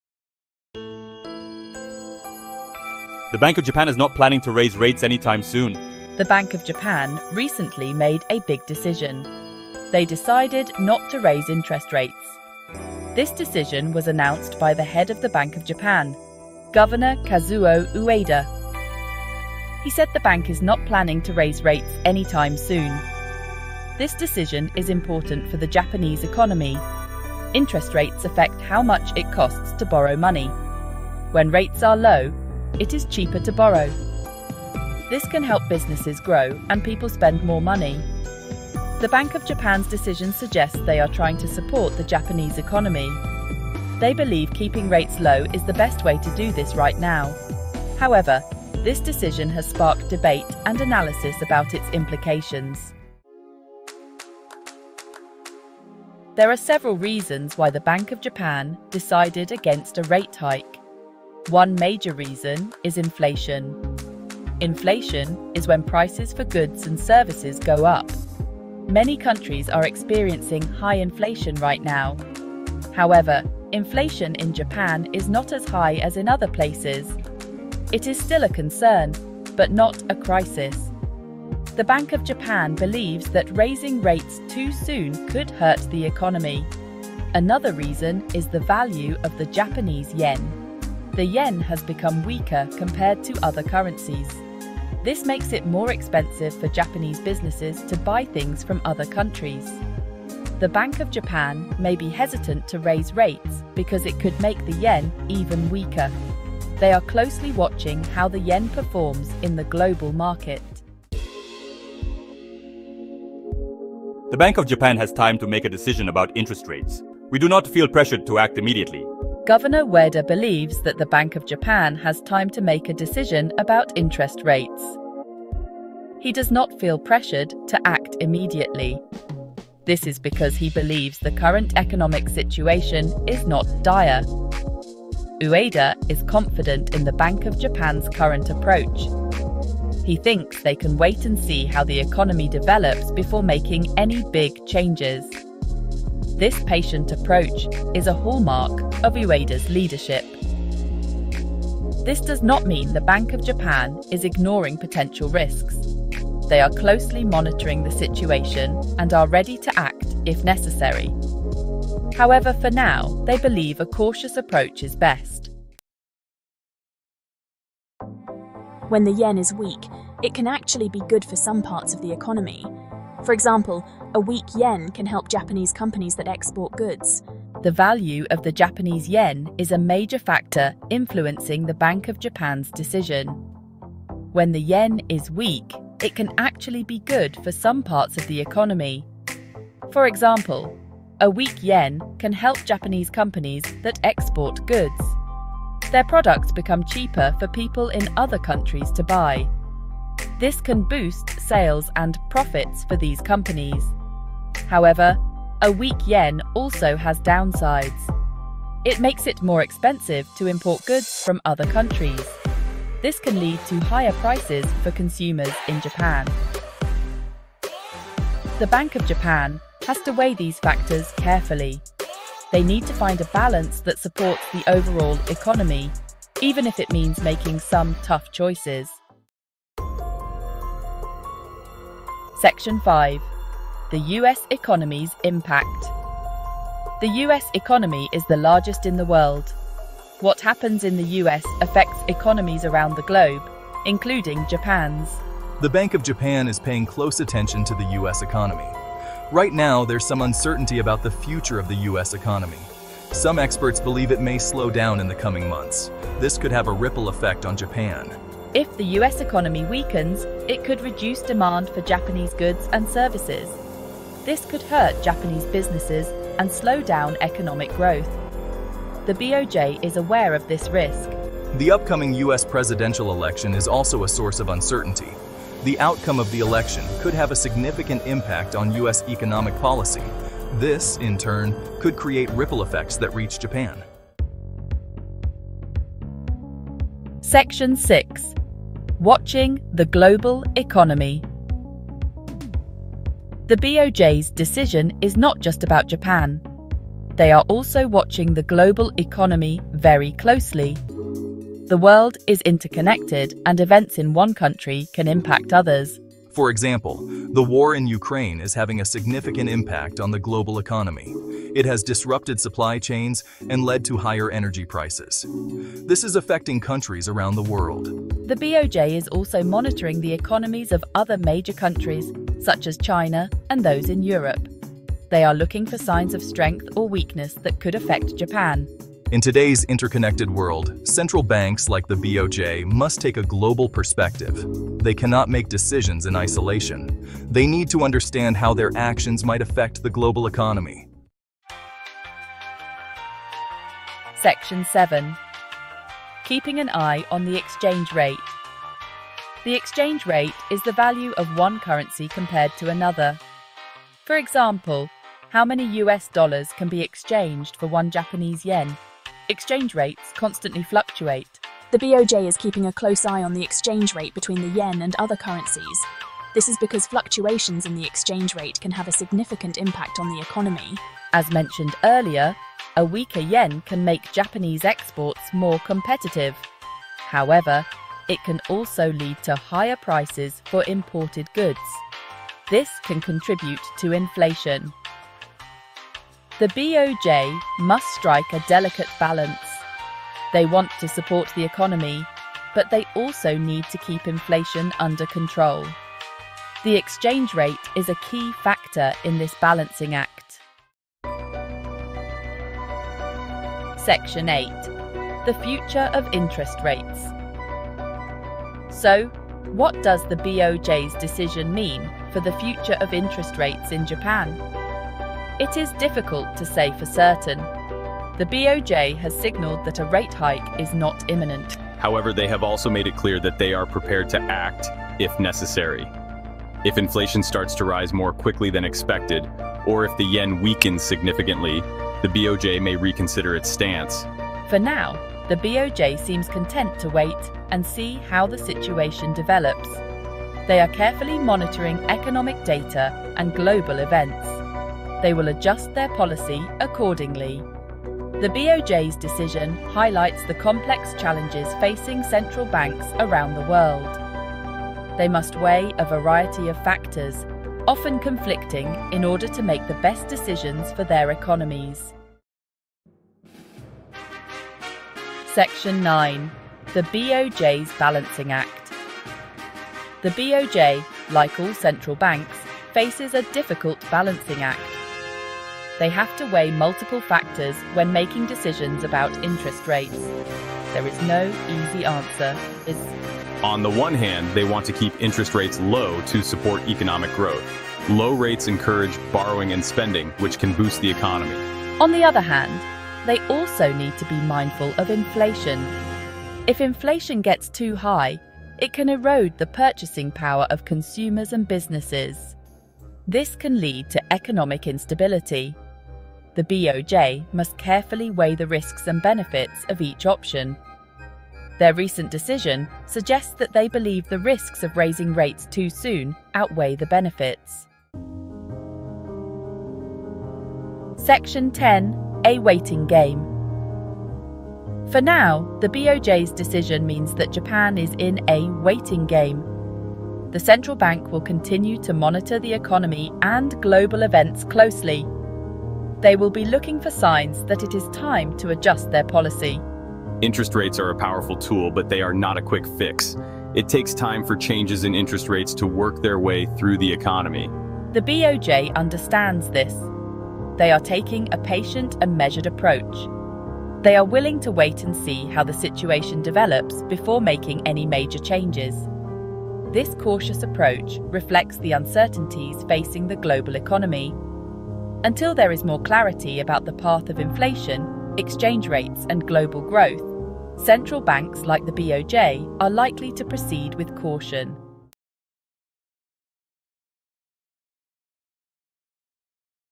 "BOJ`s Ueda: No Interest Rate Hike Imminent! "
OUTLINE:
00:00:00 The Bank of Japan's Decision
00:00:53 Why No Rate Hike?
00:01:51 Time is on Ueda's Side
00:02:45 The Yen's Influence
00:03:49 The US Economy's Impact
00:04:44 Watching the Global Economy
00:05:41 Keeping an Eye on the Exchange Rate
00:06:40 The Future of Interest Rates
00:07:42 The BOJ's Balancing Act
00:08:37 A Waiting Game

📊 **In this video, we dive into the recent comments made by BOJ Governor Kazuo Ueda regarding Japan's monetary policy!** 🇯🇵✨

🗞️ Ueda has signaled that the Bank of Japan is in no rush to hike interest rates, even as the yen hits a near three-month low. He emphasized that "time is on our side" when it comes to making crucial policy decisions. ⏳💬

🔍 **Key Highlights:**
- The importance of Ueda's statement in the current economic landscape. 📈
- Implications for the yen and Japanese markets. 💱
- Insights from Ueda's meeting with G20 counterparts in Washington. 🌐🤝

💼 Whether you're an investor, economist, or just curious about global finance, this update is essential for understanding Japan's economic strategy moving forward!